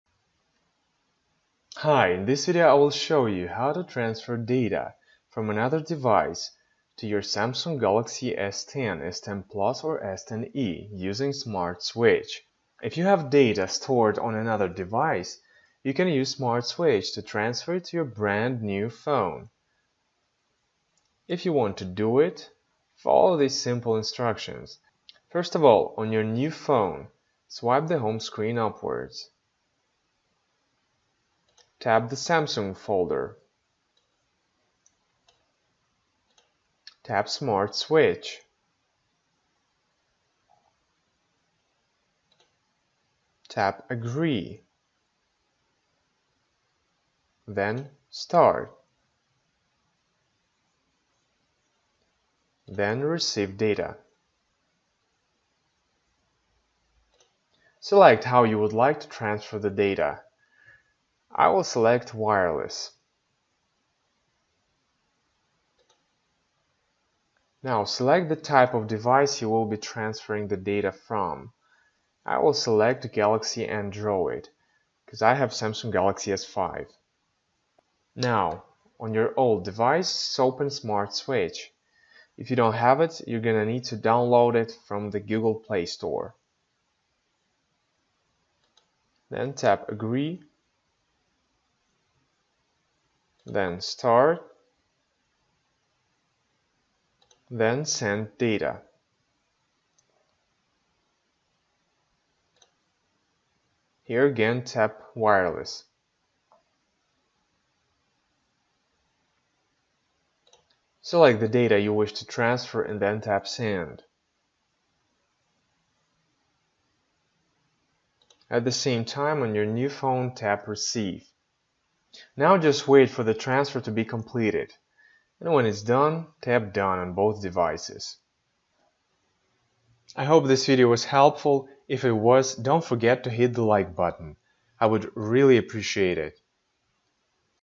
How to transfer data from your old phone to your Galaxy S10, S10 Plus or S10e
In this video, I explain how to easily transfer data from your old phone to your Galaxy S10, S10 Plus or S10e using Samsung Smart Switch